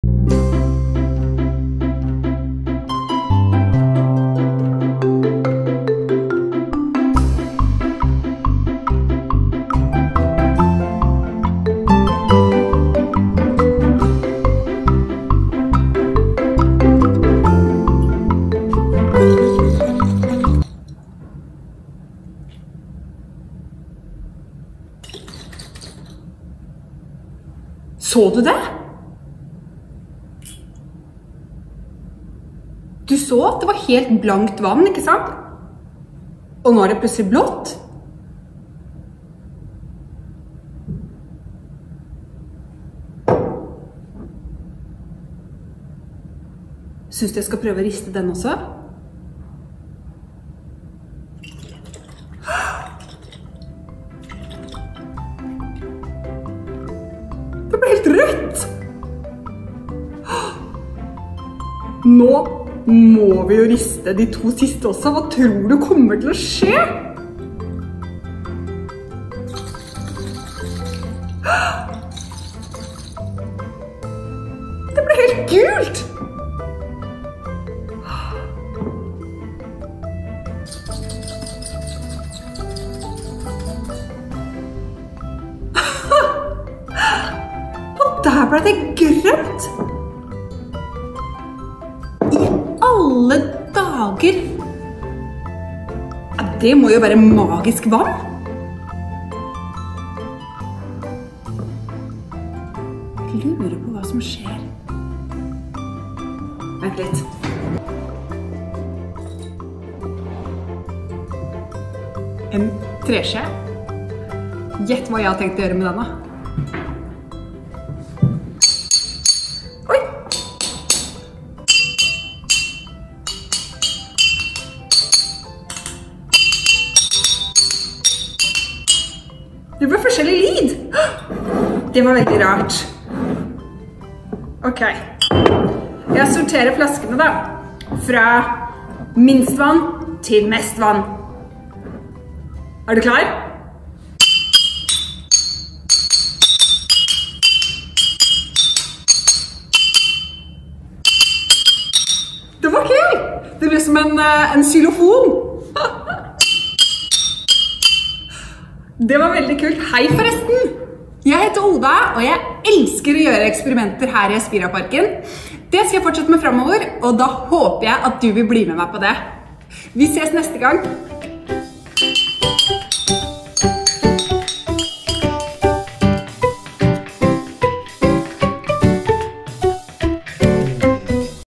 Det skjer magiske ting i Espira-parken..
Bli med meg inn i et magisk univers! Hva skjer når det plutselig står noen vannflasker på forskerbordet mitt, tror du?

Hvordan kan du lage magisk vann hjemme? Smør litt konditorfarge i korken på en flaske med vann og skru korken forsiktig på. Når du rister i flasken blander fargen seg i vannet.

Utforsk i barnehagen - vær nysgjerrig!